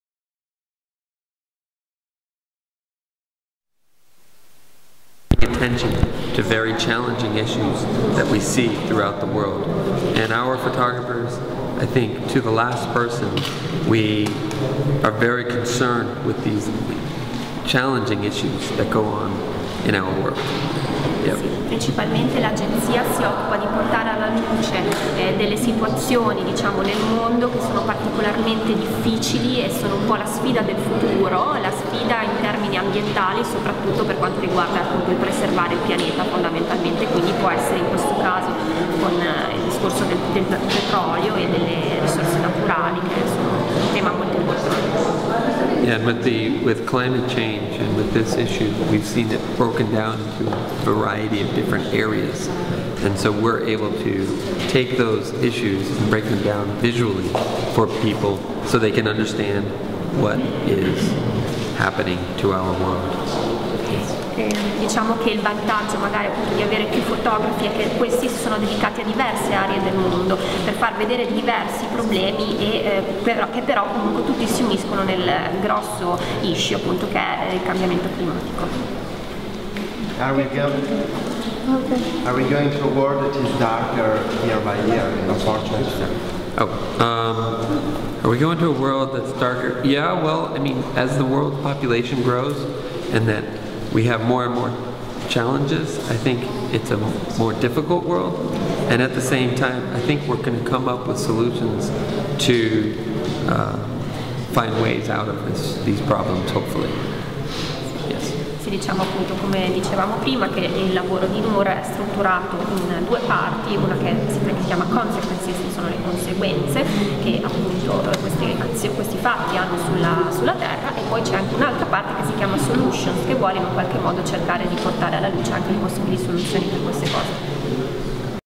Jon Lowenstein (Noor photo agency) talking about Journal on The Changing planet 's project
JOURNAL ON THE CHANGING PLANET, 2009 – 2015. E' questo il progetto che l'agenzia Noor ha portato all'edizione 2015 di Fotografia Europea, Reggio Emilia. Al centro del tema: l'uomo, la natura, le catastrofi, l'azione umana sul paesaggio e le sue conseguenze.